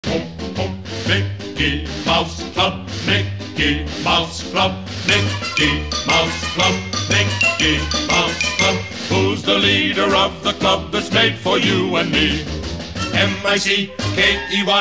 Satisfying Mickey Scratch Art
I’ve not done much scratch art before, but I thought it would be a fun and satisfying short to share with you all!
-Kaitlyn